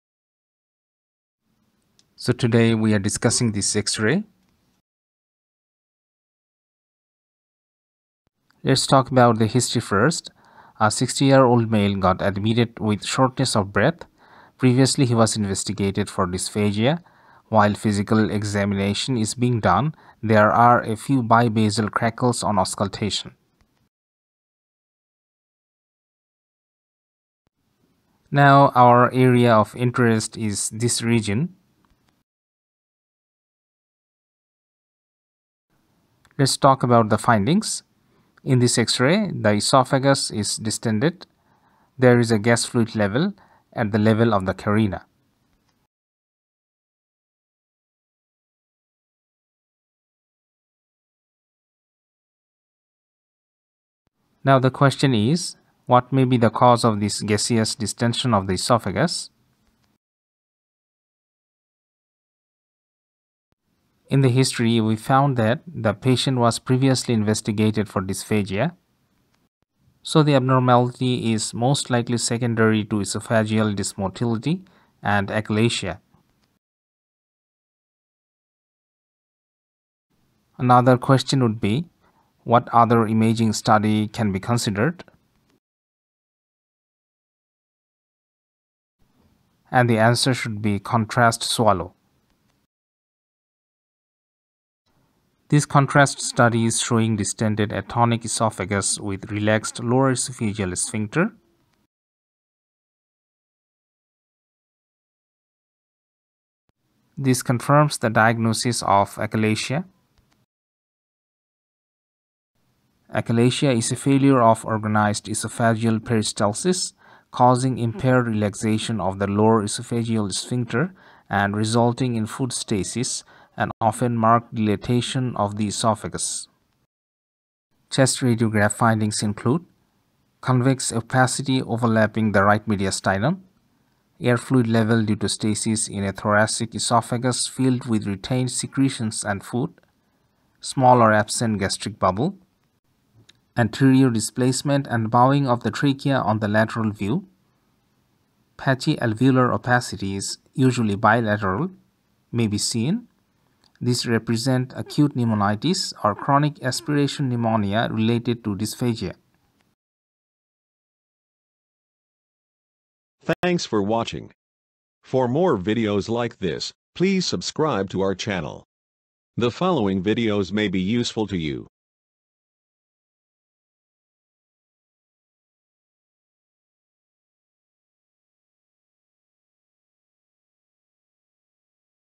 Interesting Chest X-ray #4 : Achalasia
A 60-year-old male got admitted with shortness of breath. Previously investigated for dysphagia. Few bibasal crackles on auscultation.

Findings:
Distended esophagus with gas/fluid level at the level of the carina.

What may be the cause of gaseous distention of the esophagus?
Most likely secondary to esophageal dysmotility and achalasia.

What other imaging study would you consider?
Contrast swallow.

Barium swallow:
Distended atonic esophagus with relaxed lower esophageal sphincter.

Achalasia is a failure of organized esophageal peristalsis causing impaired relaxation of the lower esophageal sphincter, and resulting in food stasis and often marked dilatation of the esophagus.

Chest radiograph findings include:
Convex opacity overlapping the right mediastinum.
Air-fluid level due to stasis in a thoracic esophagus filled with retained secretions and food.
Small or absent gastric bubble.
Anterior displacement and bowing of the trachea on the lateral view.
Patchy alveolar opacities, usually bilateral, may be seen. These represent acute pneumonitis or chronic aspiration pneumonia related to dysphagia.

Reference:
Case courtesy of Dr Derek Smith, Radiopaedia.org, rID: 49713